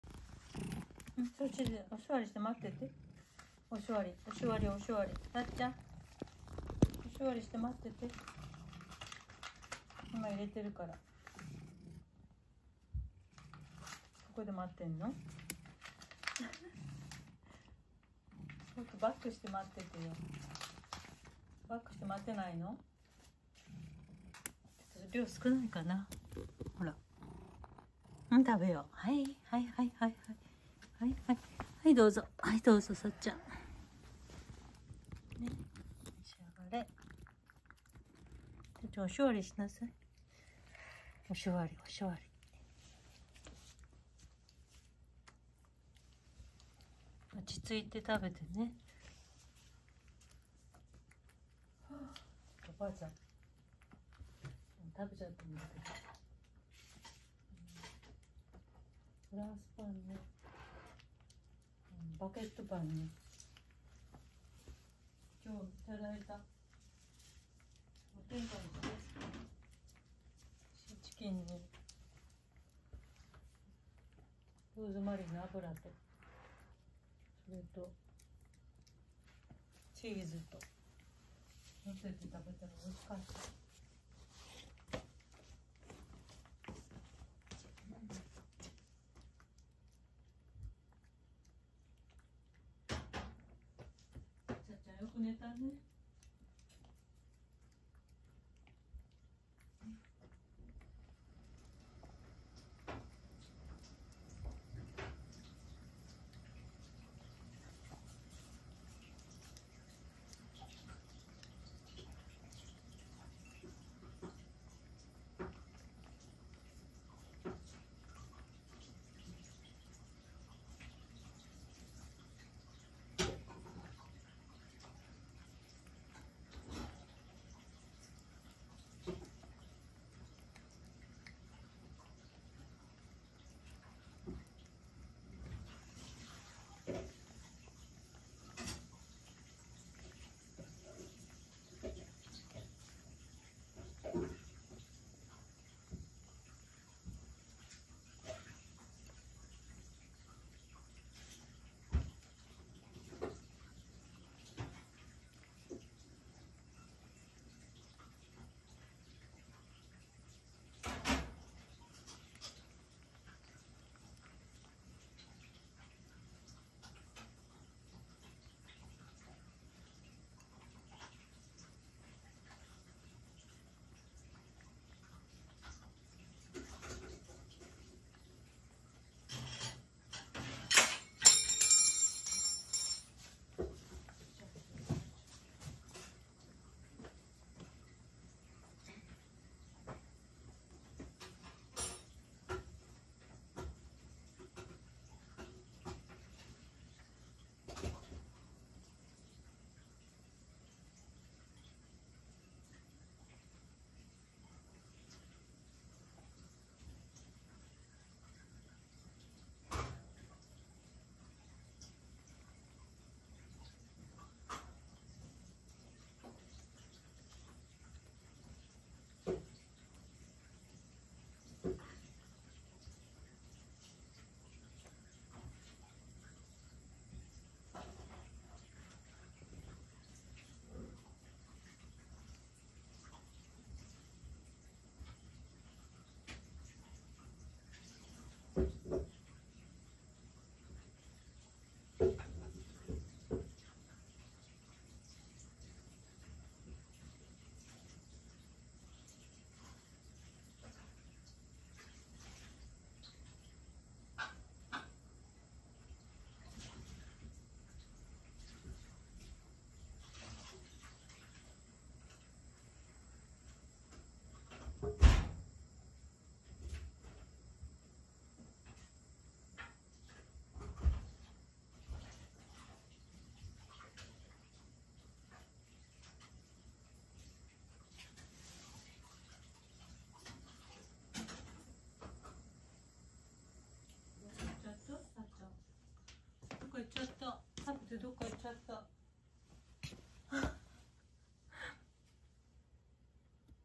楕円のベッドから起きて来て.. ウエットフードを完食して.. いつのまにか居なくなっていた猫・・・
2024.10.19(土)のさっちゃんです

　かぜのねこ

　かぜのねこ